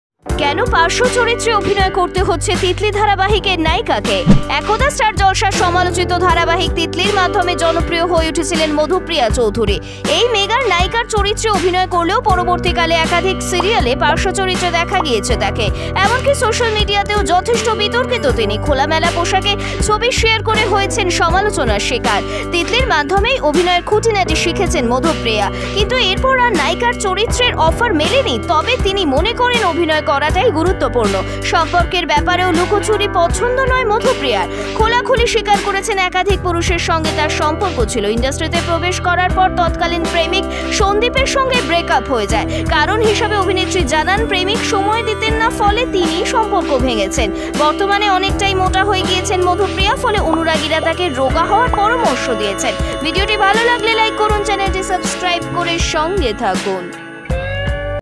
কেন পার্শ্বচরিত্রে অভিনয় করতে হচ্ছে 'তিতলি' ধারাবাহিকের নায়িকাকে!
একদা স্টার জলসার সমালোচিত ধারাবাহিক ‘তিতলি’-র মাধ্যমে জনপ্রিয় হয়ে উঠেছিলেন মধুপ্রিয়া চৌধুরী। এই মেগায় নায়িকার চরিত্রে অভিনয় করলেও পরবর্তীকালে একাধিক সিরিয়ালে পার্শ্ব চরিত্রে দেখা গিয়েছে তাঁকে। এমনকি সোশ্যাল মিডিয়াতেও যথেষ্ট বিতর্কিত তিনি। খোলামেলা পোশাকে ছবি শেয়ার করে হয়েছেন সমালোচনার শিকার। ‘তিতলি’-র মাধ্যমেই অভিনয়ের খুঁটিনাটি শিখেছেন মধুপ্রিয়া। কিন্তু এরপর আর নায়িকার চরিত্রের অফার মেলেনি। তবে তিনি মনে করেন, অভিনয় করাটাই গুরুত্বপূর্ণ। সম্পর্কের ব্যাপারেও লুকোচুরি পছন্দ নয় মধুপ্রিয়ার। খোলাখুলি স্বীকার করেছেন, একাধিক পুরুষের সঙ্গে তাঁর সম্পর্ক ছিল। ইন্ডাস্ট্রিতে প্রবেশ করার পর তৎকালীন প্রেমিক সন্দীপের সঙ্গে ব্রেকআপ হয়ে যায়।